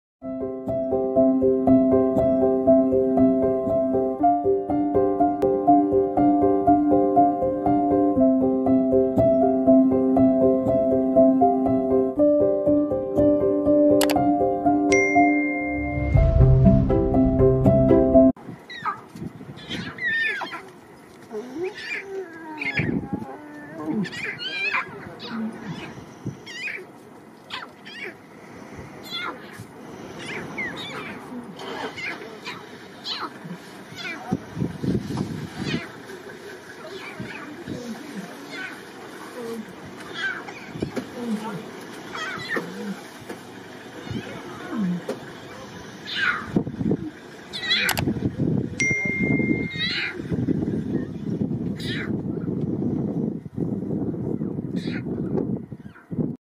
Real Audio How Lion Cub Communicating With Mother | SonikaBalaji | Tamil | தமிழ்
In this video I was showed how cute Little Lion Cub Communicating with mother , lets see and enjoy it

This videos was Taken From Creative Common license -attribution ( Reuse Allowed)